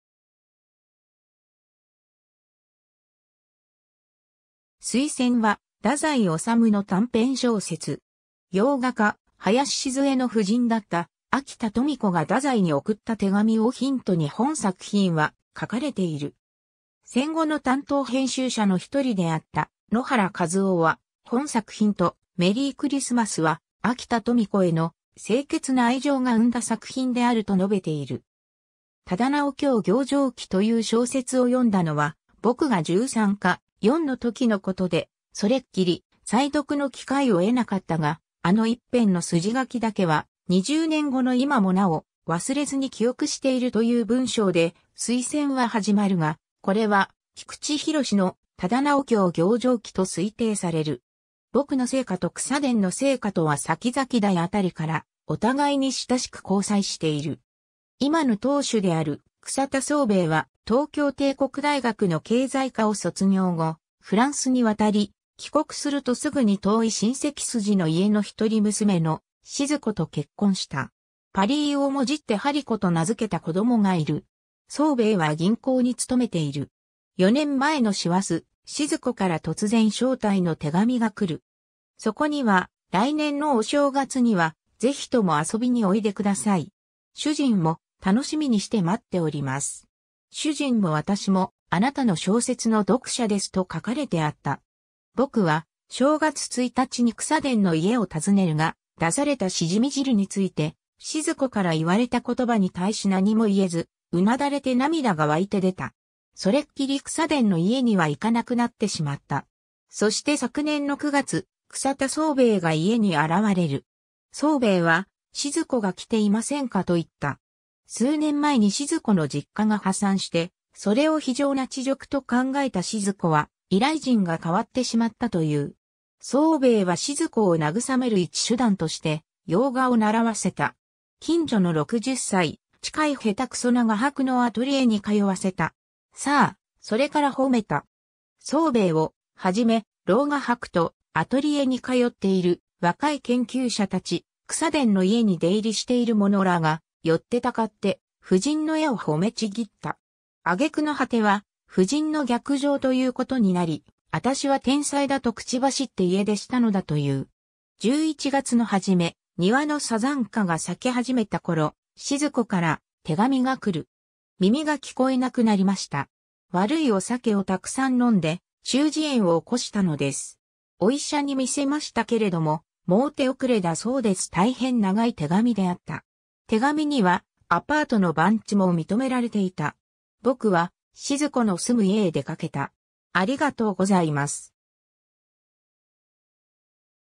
水仙 (小説)
改造_(雑誌)_掲載の小説
「水仙」（すいせん）は、太宰治の短編小説。
 洋画家・林倭衛の夫人だった秋田富子が太宰に送った手紙をヒントに本作品は書かれている。
 戦後の担当編集者の一人であった野原一夫は、本作品と「メリイクリスマス」は秋田富子への「清潔な愛情が生んだ作品である」と述べている。
 「『忠直卿行状記』という小説を読んだのは、僕が十三か、四のときの事で、それっきり再読の機会を得なかったが、あの一篇の筋書だけは、二十年後のいまもなお、忘れずに記憶している」という文章で『水仙』は始まるが、これは菊池寛の『忠直卿行状記』と推定される。
 「僕」の生家と草田の生家とは先々代あたりからお互いに親しく交際している。
今の当主である草田惣兵衛は東京帝国大学の経済科を卒業後、フランスに渡り、帰国するとすぐに遠い親戚筋の家のひとり娘の静子と結婚した。
パリイをもじって玻璃子（はりこ）と名づけた子供がいる。
惣兵衛は銀行に勤めている。
 4年前の師走、静子から突然招待の手紙が来る。
そこには「来年のお正月には、ぜひとも遊びにおいで下さい。主人も、たのしみにして待っております。主人も私も、あなたの小説の読者です」と書かれてあった。
 「僕」は正月一日に草田の家を訪ねるが、出された蜆汁について静子から言われた言葉に対し何も言えず、うなだれて涙が沸いて出た。
それっきり草田の家には行かなくなってしまった。
 そして昨年の9月、草田惣兵衛が家に現れる。
惣兵衛は「静子が来ていませんか」と言った。
数年前に静子の実家が破産して、それを非常な恥辱と考えた静子は、以来人が変わってしまったという。
惣兵衛は静子を慰める一手段として洋画を習わせた。
近所の60歳近い下手くそな画伯のアトリエに通わせた。
さあ、それから褒めた。
惣兵衛をはじめ、老画伯とアトリエに通っている若い研究者たち、草田の家に出入りしている者らが、寄ってたかって夫人の絵を褒めちぎった。
あげくの果ては夫人の逆上ということになり、「あたしは天才だ」と口走って家出したのだという。
 11月のはじめ、庭の山茶花が咲き始めた頃、静子から手紙がくる。
 「耳が聞えなくなりました。悪いお酒をたくさん飲んで、中耳炎を起したのです。お医者に見せましたけれども、もう手遅れだそうです」 たいへん長い手紙であった。
手紙には、アパートの番地も認められていた。
「僕」は静子の住む家へ出かけた。